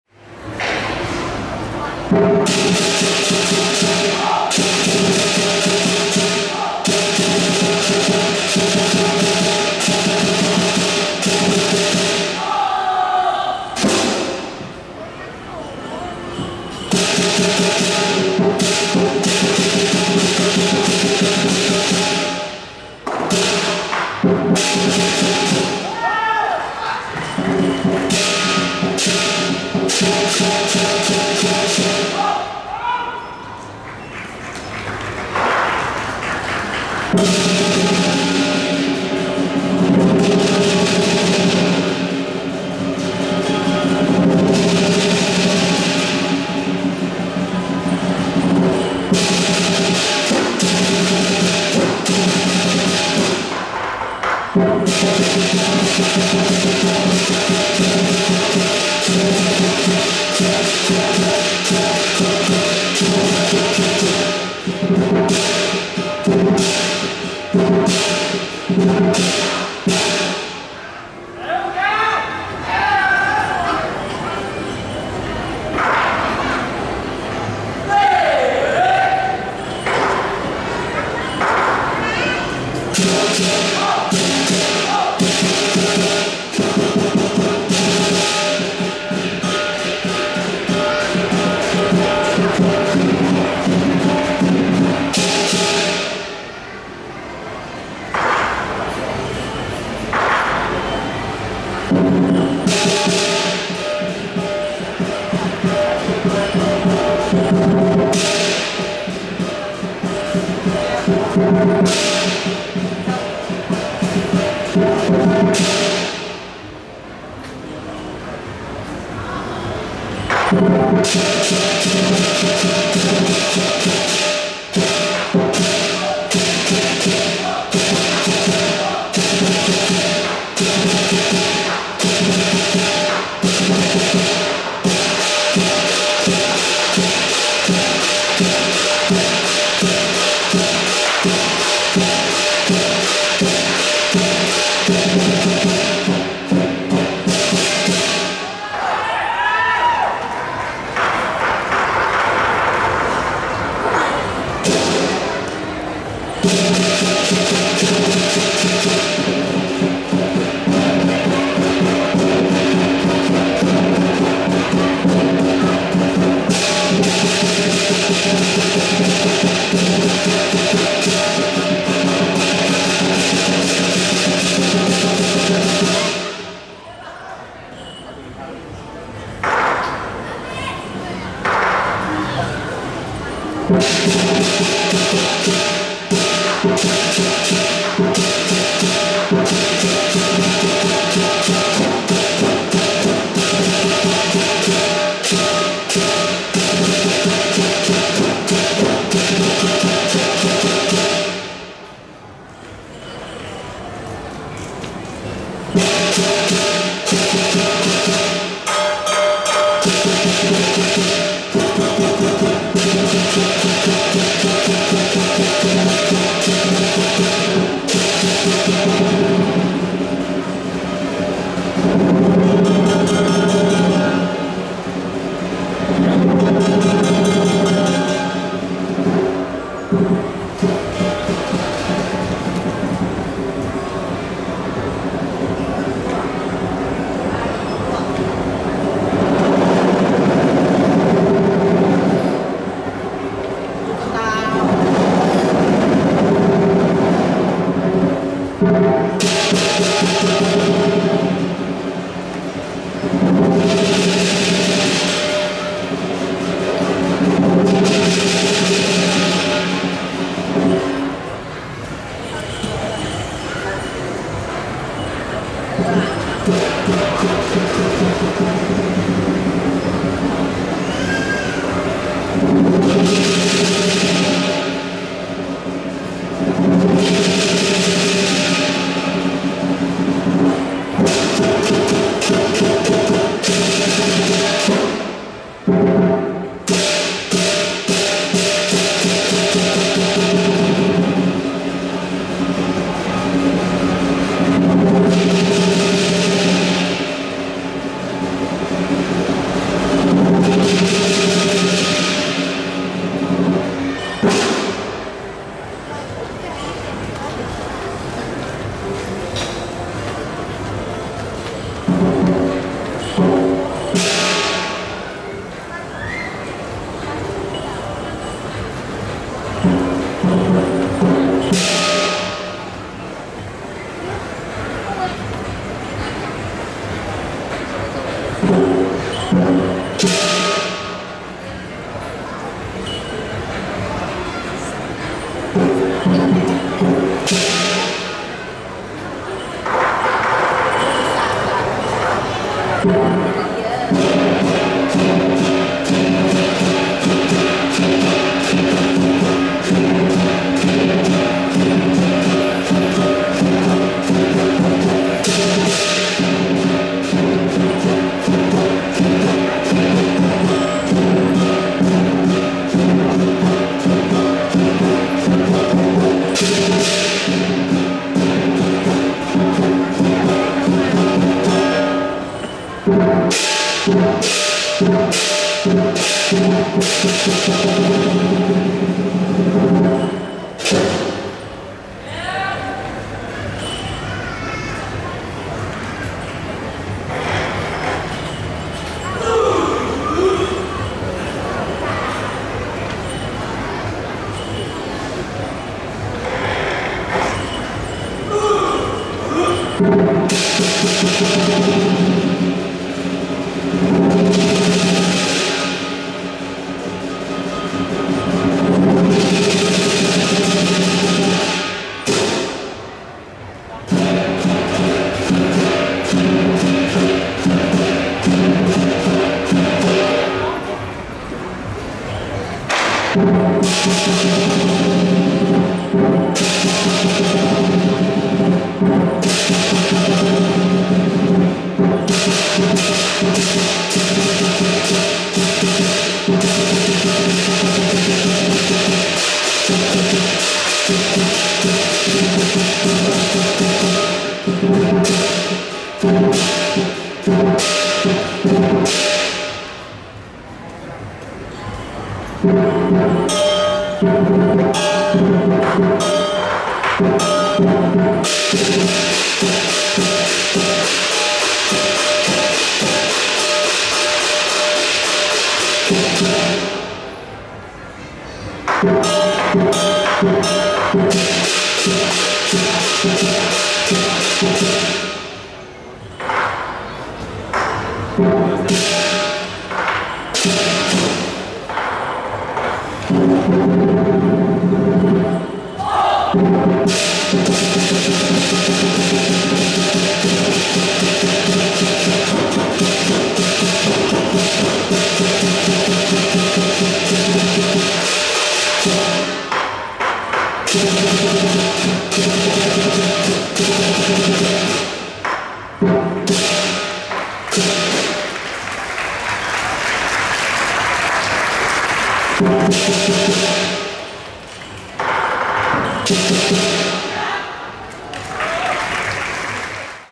Lion Dance by Puchong Feng Yang 蒲种峰阳 - Traditional Lion Dance Championship @ Cheras Leisure Mall
(7/5/2023)

Puchong Feng Yang Dragon & Lion Dance Troupe - 2023 4th Teratai Cup Traditional Lion Dance Championship @ Cheras Leisure Mall

雪州蒲种峰阳龙狮团 - 2023 年第四届全国传统南狮狮王争霸赛 (莲花苑杯) @ Cheras Leisure Mall

Deduction 扣除 : 0.10
Final Score 得分 : 8.46